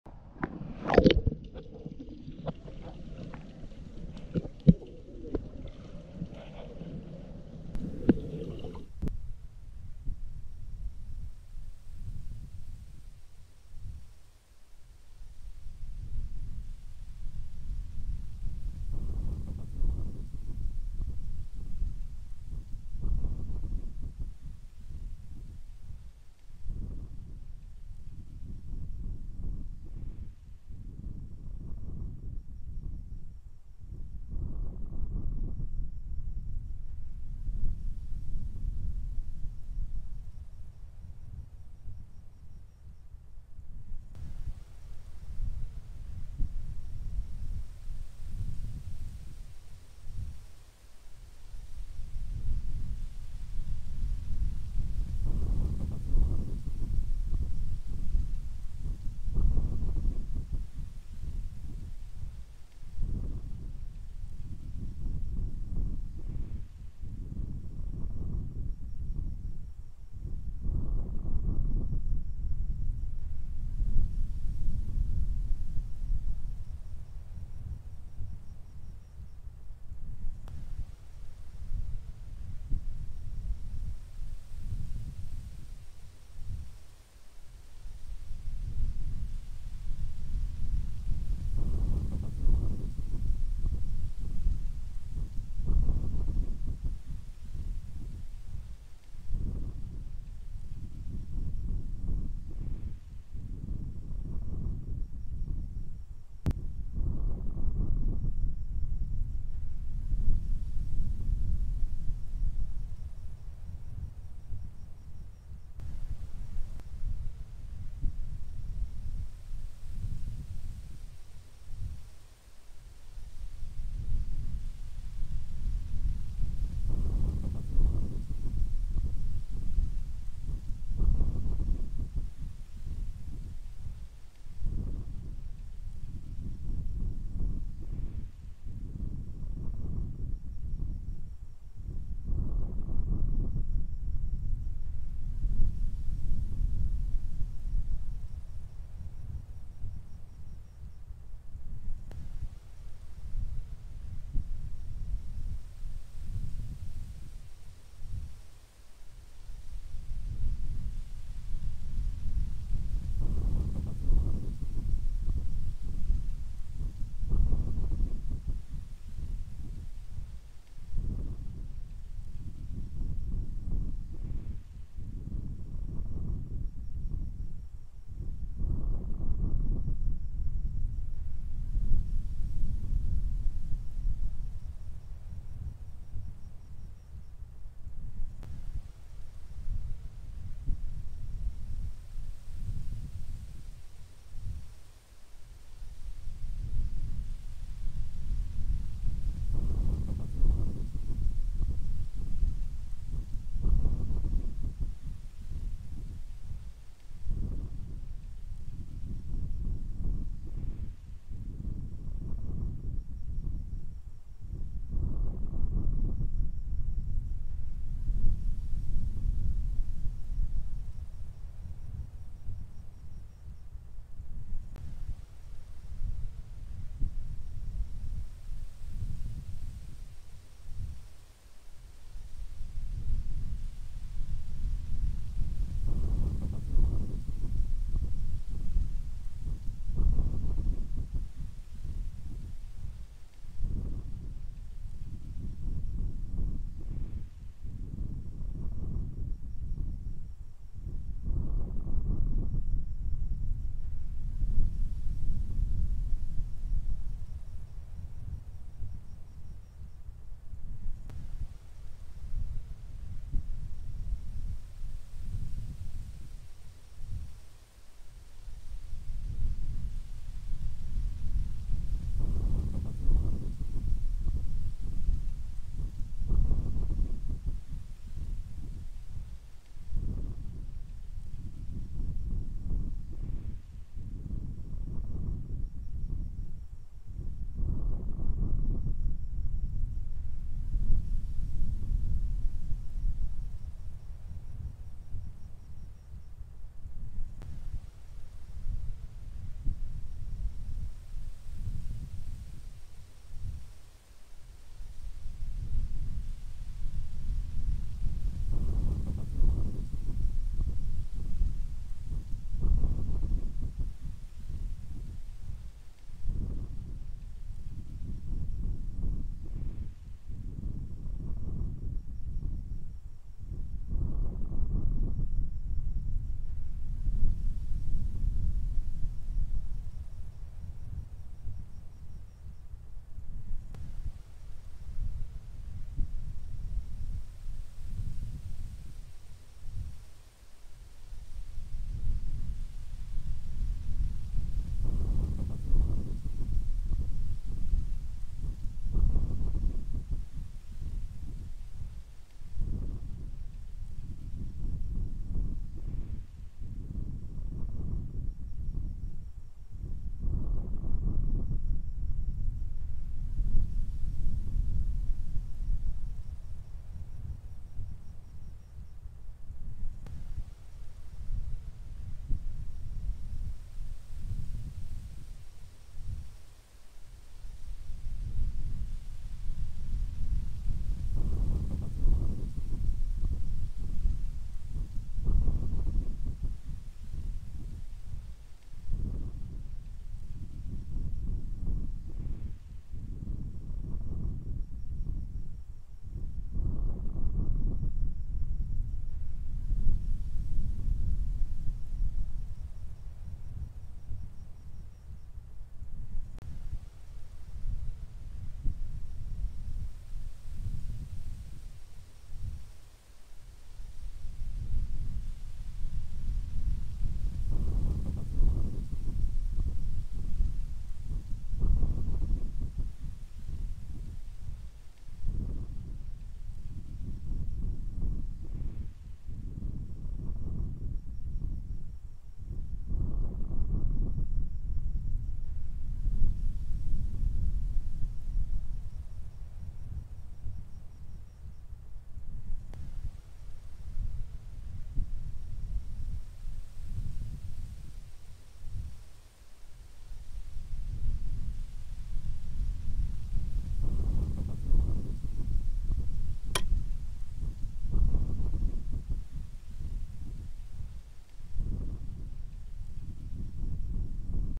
[4K] Nature Walk: Vayang Rolling Hills, Basco, Batanes, Philippines
Have a look at breath taking scenic view of the Vayang rolling hills overlooking Mount Iraya, Basco, Batanes, Philippines. A road leads up to a trail where it's an easy five-minute walk to views over rocky beach and the endless, green Vayang Rolling Hills. If the weather is dreary you'll feel more like you're in Scotland than in the Philippines.